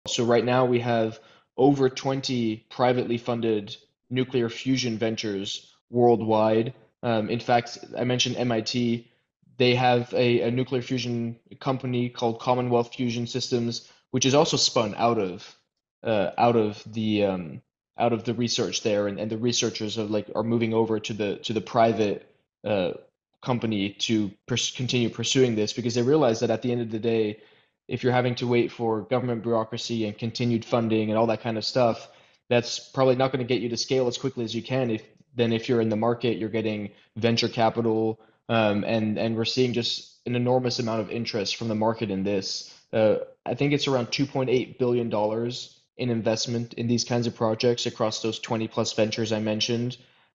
Market interest in nuclear fusion is growing
There are now over 20 privately funded nuclear fusion ventures worldwide with a total of around $2.8 billion in investment.